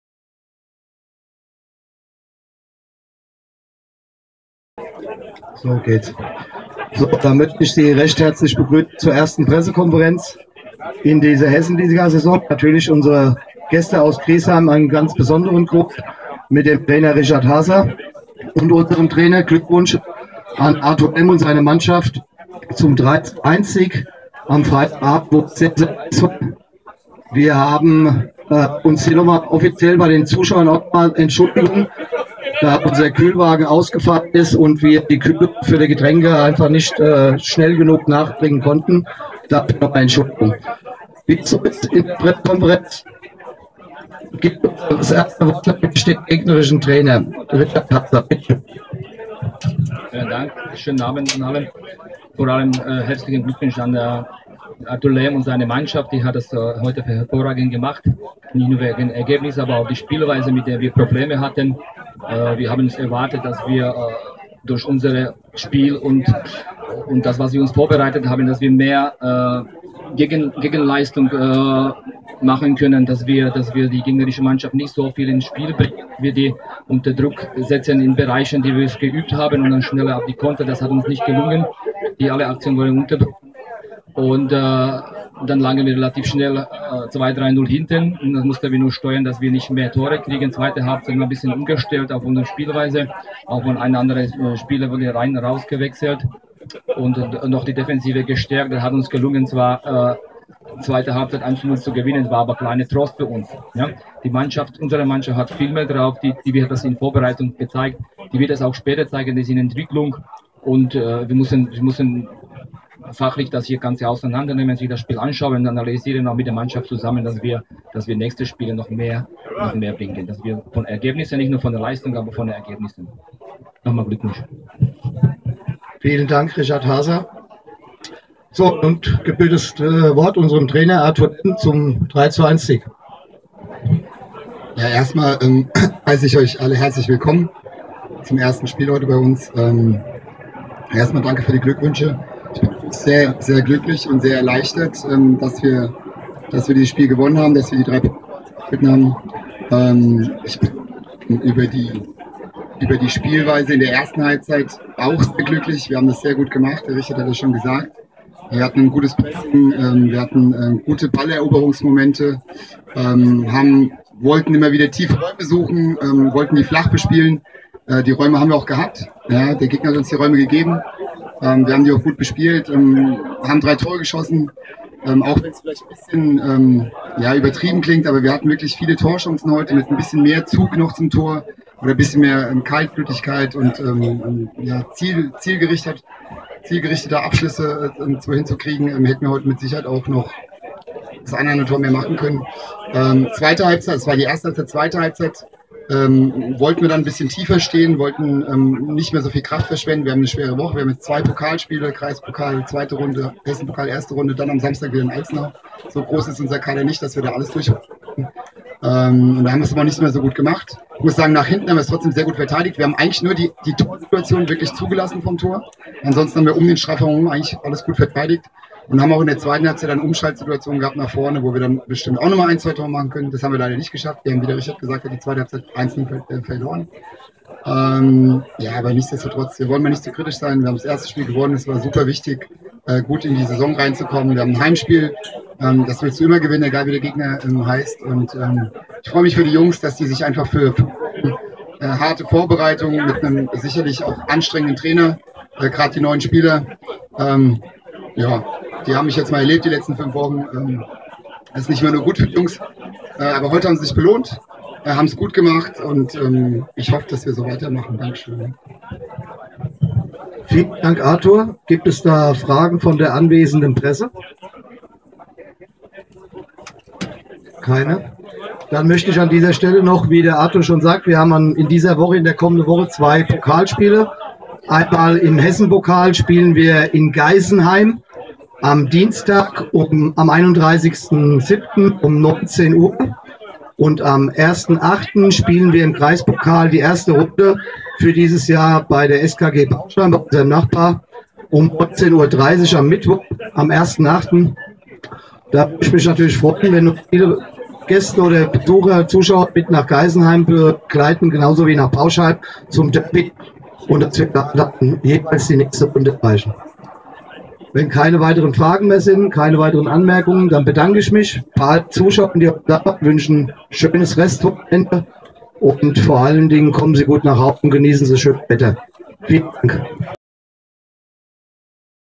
PK zum Spiel VfB Ginsheim - SC Vikt. Griesheim
Nach dem ersten Heimspiel der HL-Saison 2018/2019 zwischen dem VfB Ginsheim und dem SC Vikt. Griesheim folgte im Anschluss die PK